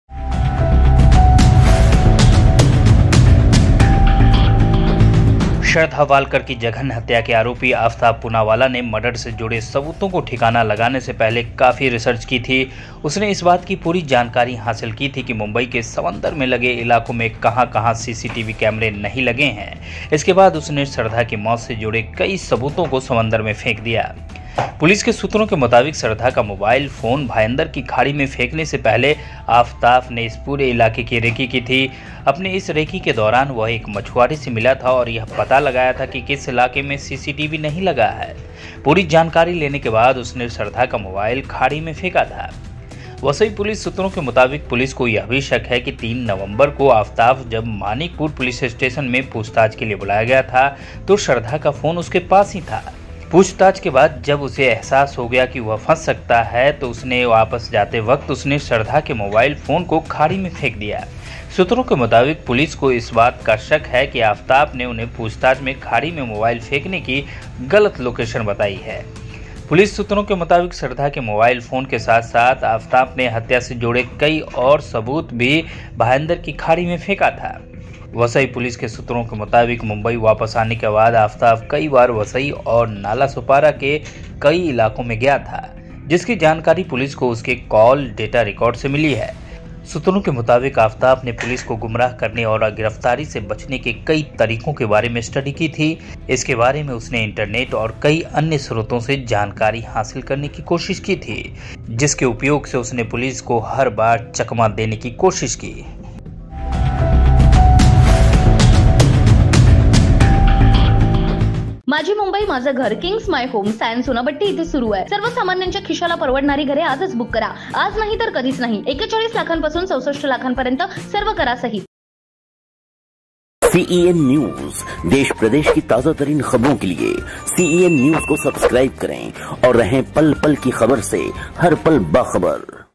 आफताब ने श्रद्धा का फोन फेंकने से पहले की थी रेकी, मछुआरे से ली CCTV कैमरों की जानकारी
Shraddha Murder Case: आफताब ने श्रद्धा का फोन फेंकने से पहले की थी रेकी, मछुआरे से ली CCTV कैमरों की जानकारी